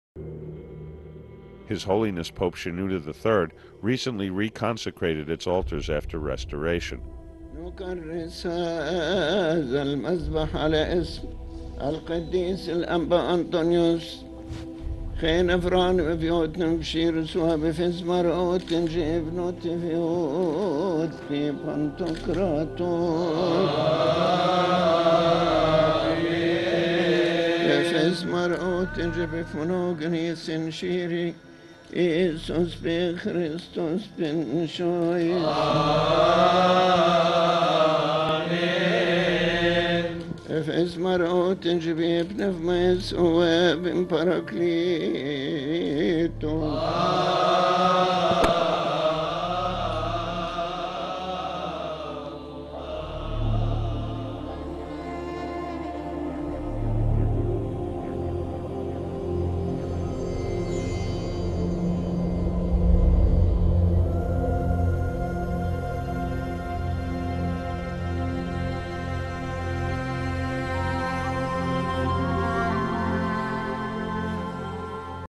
Re-consecration of St. Antony's Ancient Church | 2003 A.D
Re-consecration of St Antony's Ancient Church at St Antony's Monastery, Egypt
In the hands of His Holiness Pope Shenouda III
Jan - 2003

إعادة تدشين كنيسة القديس العظيم الأنبا أنطونيوس الآثرية بدير القديس الأنبا أنطونيوس، مصر
بيد قداسة البابا شنودة الثالث
يناير - 2003